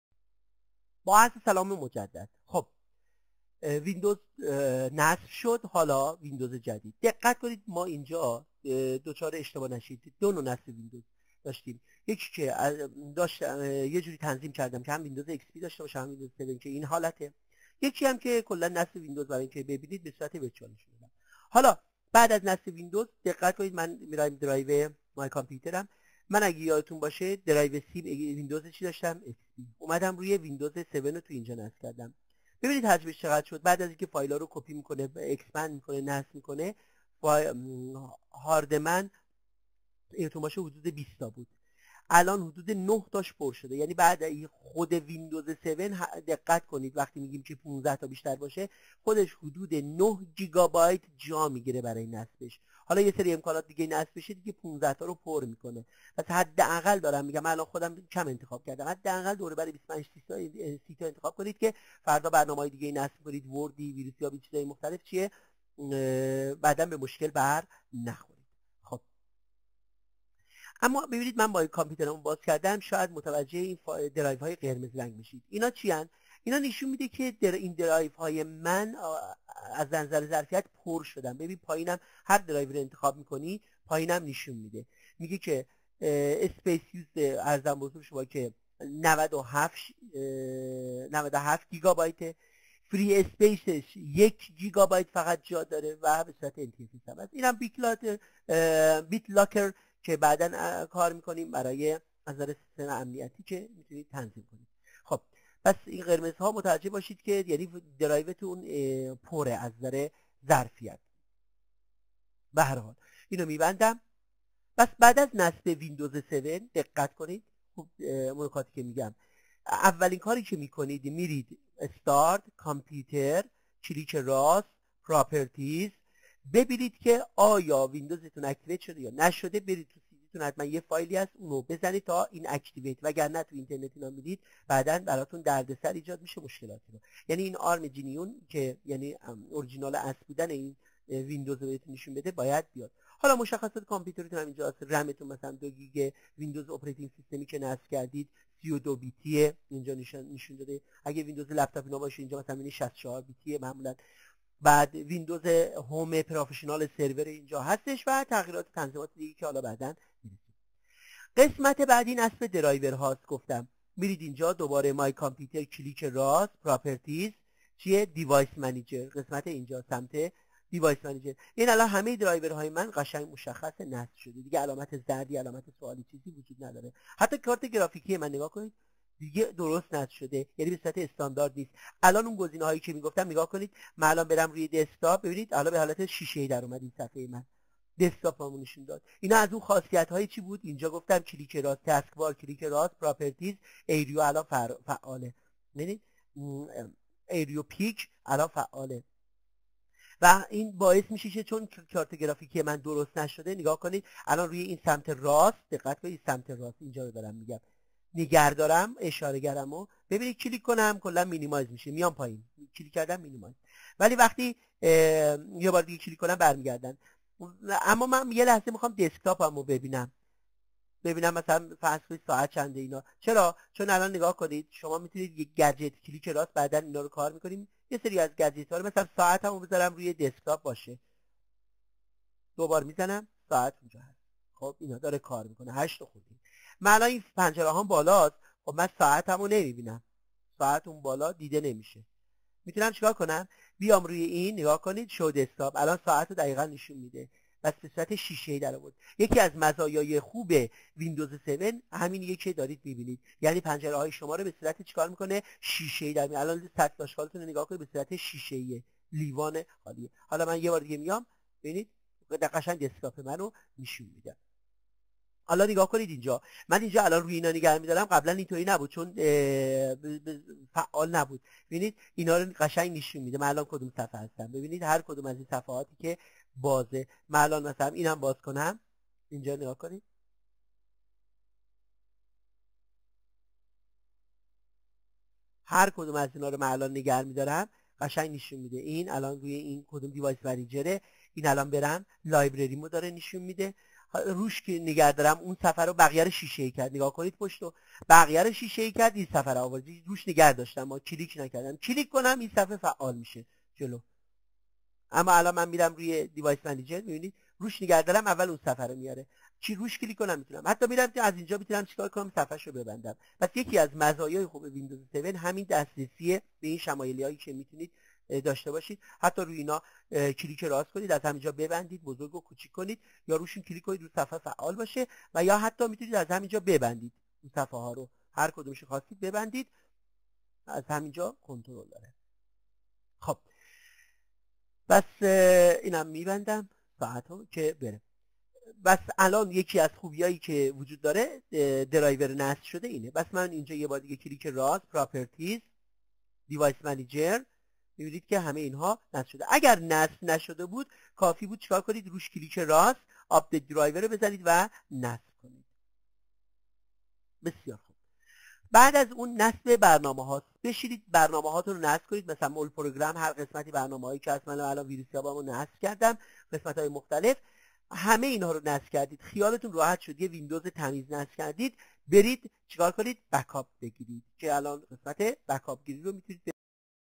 11-After-Install-2-WindowsSeven-AkbarZahiri
آموزش کامل - قدم به قدم نصب، راه اندازی، تنظيم و کار با تمامی قسمتهای ويندوز ايکس پی  و کامل تر از همه ويندز سون7 شامل نصب برنامه ها، درایورها و همچنین اينترنت 8 و ده ها برنامه ديگر از جمله شبکه و کنترل از راه دور و نحوه کنترل فرزندان و مهمتر از همه بابای بچه ها که بیشتر از همه احتیاج به کنترل دارد
توجه شود هرچند این ویندوزها اکنون قدیمی می باشند ولی تقریبا نحوه تنظیم و کار آنها شبیه و یکسان می باشد و مهمتر از همه نحوه تدریس به صورتی هست که  بیشتر مطالب موزد نیاز کار با ویندوزهای جدید را نیز پوشش می دهد
Windows Seven 7 and  Windows XP Installation and Settings